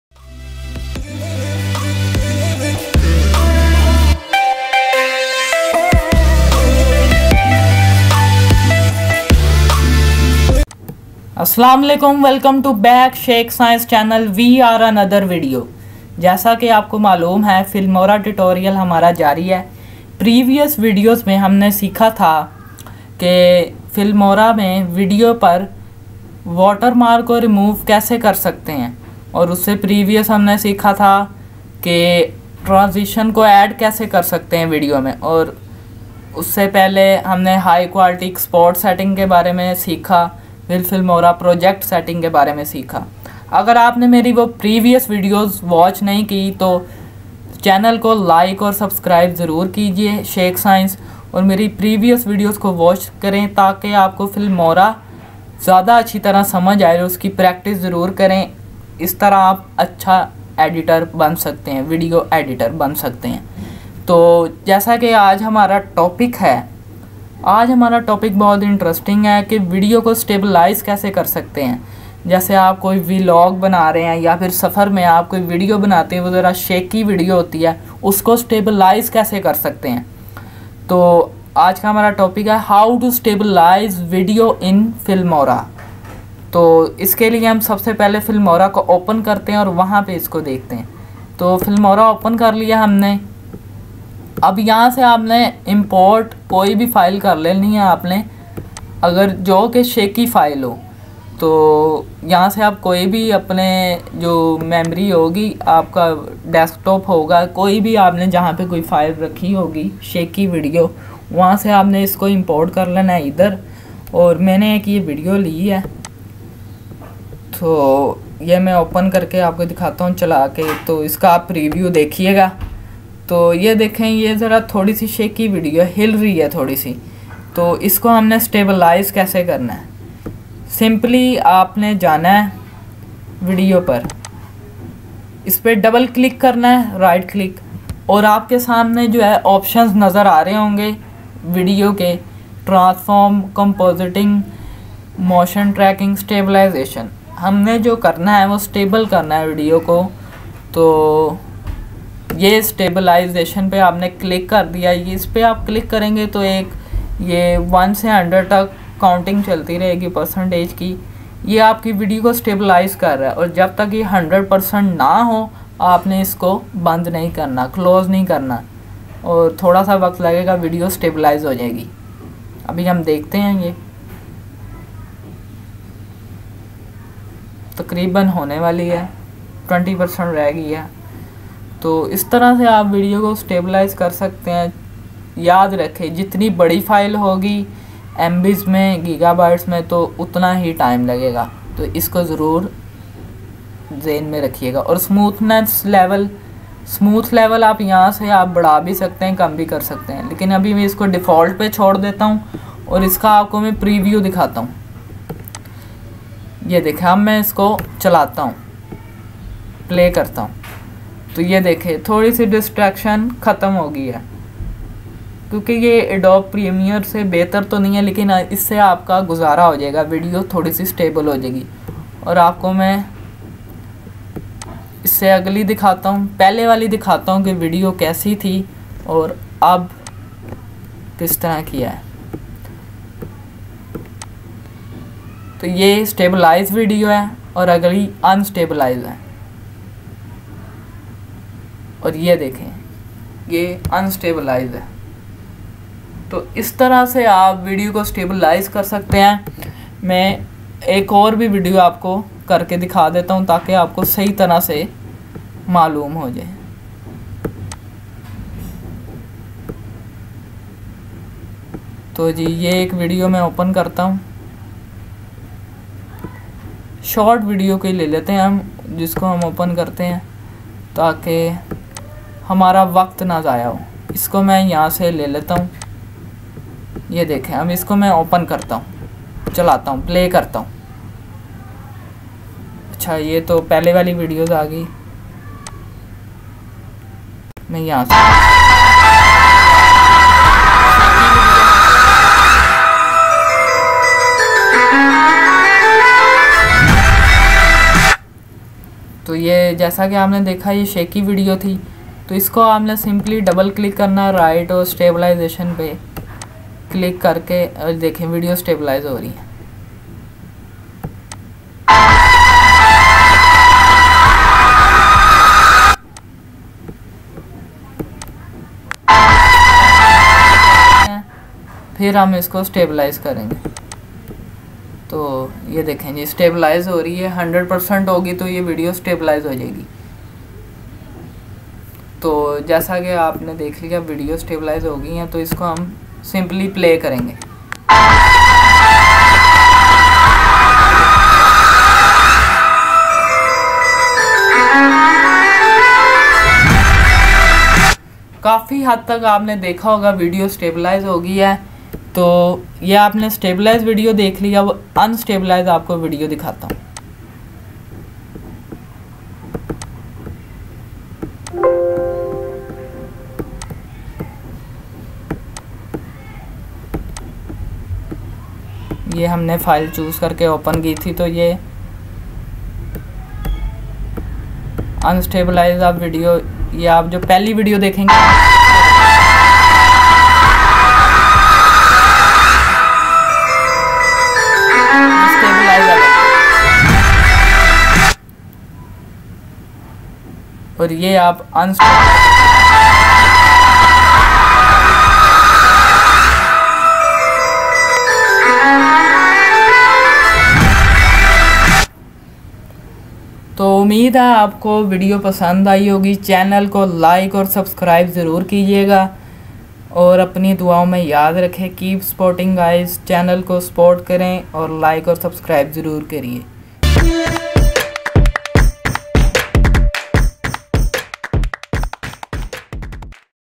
How to Stabilize Shaky video in Filmora 12 | Filmora 2023
This video shows you how to stabilize your shaky videos using Wondershare Filmora 12

In this video we have provided a step by step procedure on how to stabilizing the shaky video in Filmora video editing software

Don't miss out on our tutorial that will have you editing like a pro in no time so watch now to quickly stabilize any shaky footage with ease!

How to stabilize Shaky video in Filmora 12 - Tutorial for beginners